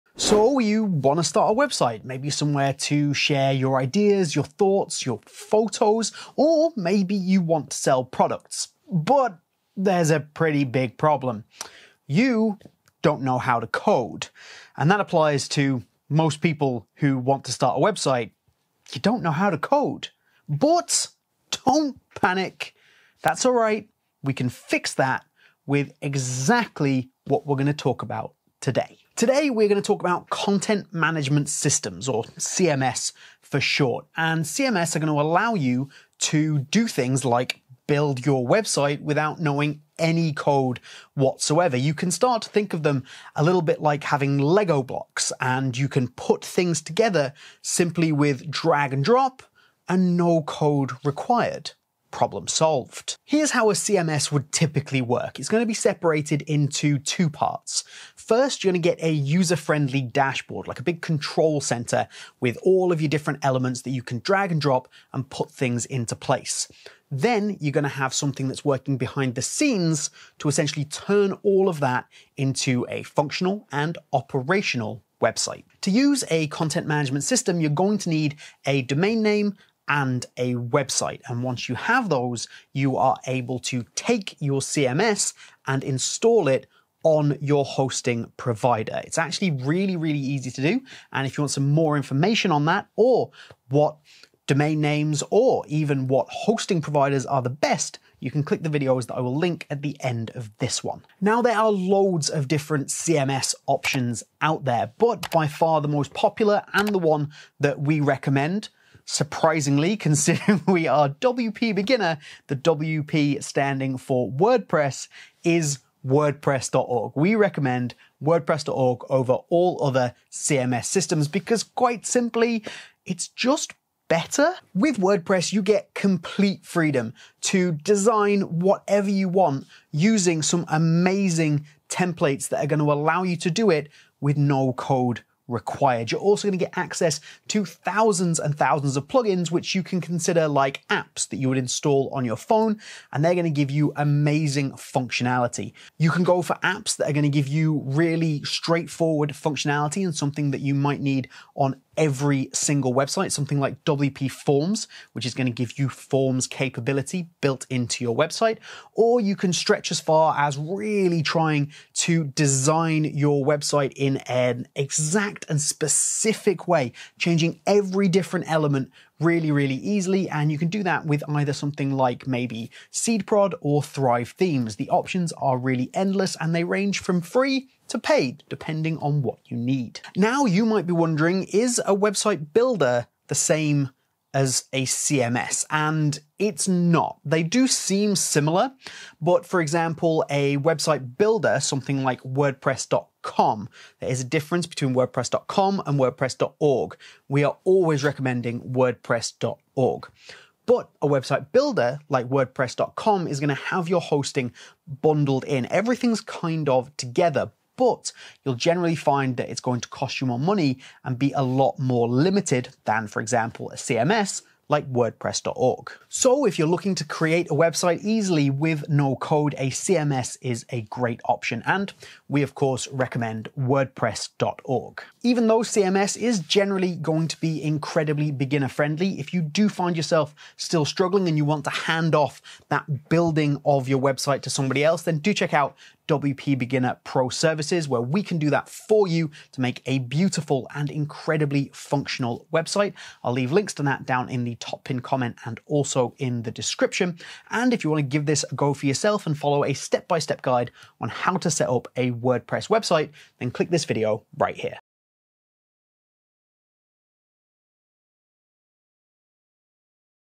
What is a CMS
What is a CMS? Something that you're going to need to know yet can sound a little bit daunting, however, no need to worry, we are going to break down what is a CMS for you today, by the end of the video, you'll be a CMS expert and you'll know which CMS, or content management system is right for you.

If you want to check out WPForms as mentioned in the video then

If you need a hand from the professionals then

Top Resources